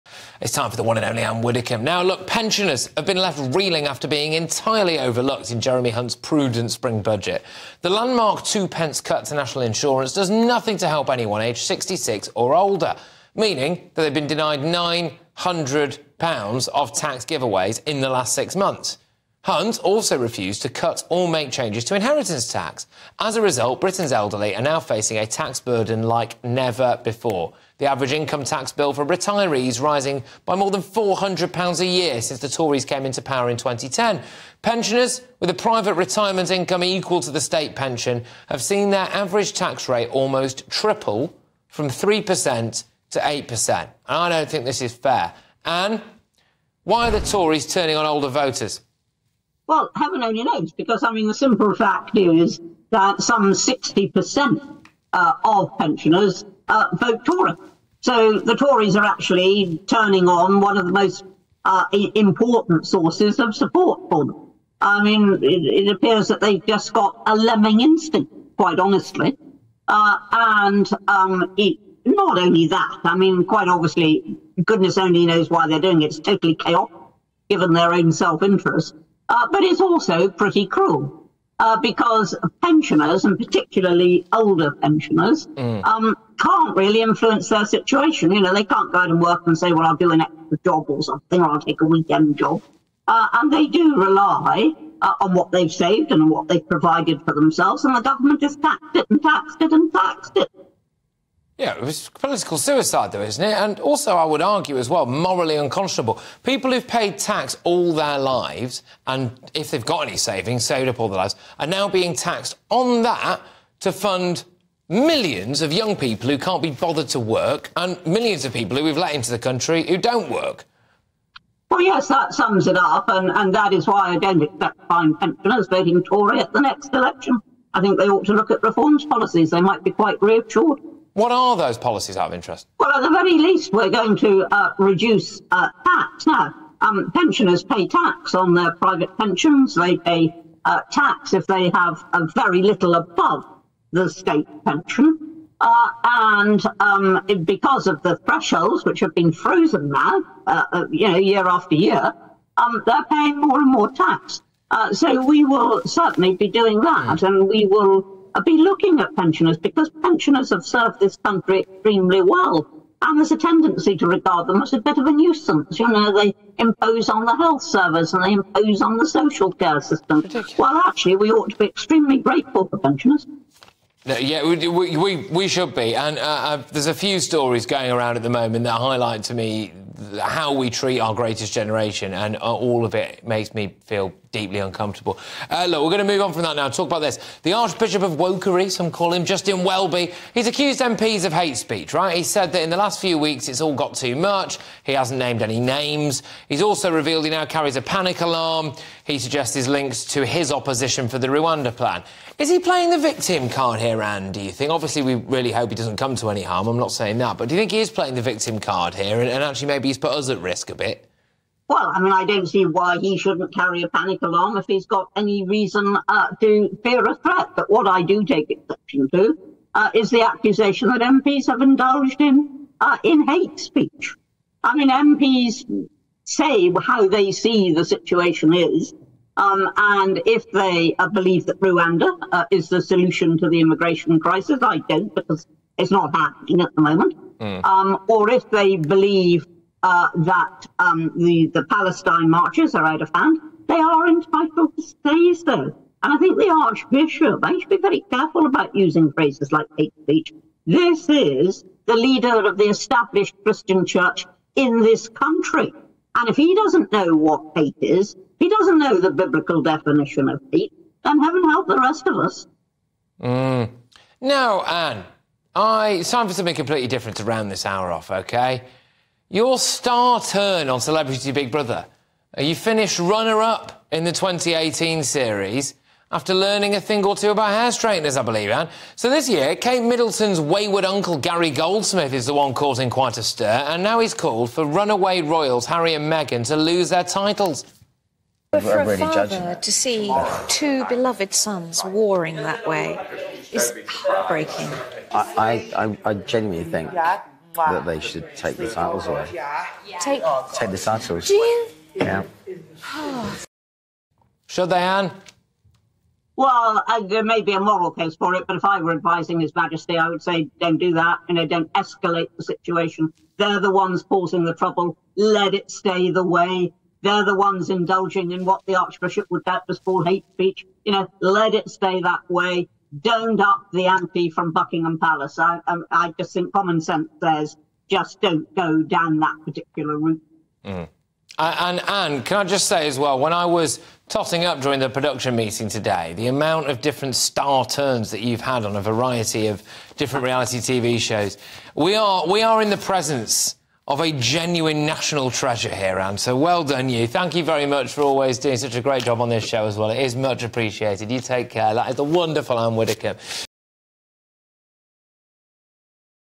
Tory's 'pretty CRUEL' Budget means pensioners should 'look at Reform UK's policies' - Ann Widdecombe
'They ought to look at Reform's policies, they might be quite reassured'.

Former Conservative MP, Ann Widdecome, says pensioners who feel the Tories have turned their back on them should look into Reform UK.